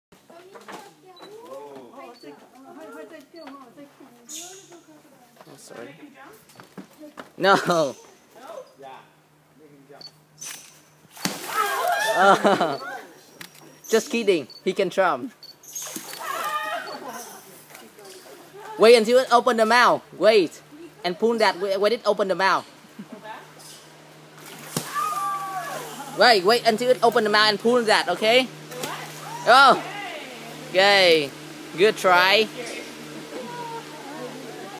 Heather the Croc Feeder!
Feeding a crocodile on Can Gio Island, Vietnam.